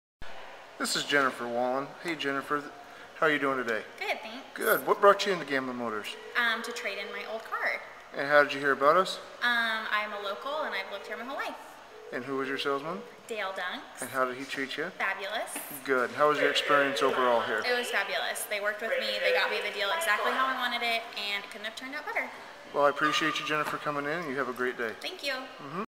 Jennifer Wallin 2011 Chevrolet Equinox Dale Dunks
Jennifer Wallin Proudly supported her local car dealership when she purchased a 2011 Chevy Equinox. Jennifer and the Wallin Family have been here in Enumcalw for many years. The Wallin's are a big party of the farming and Dairy communtiy.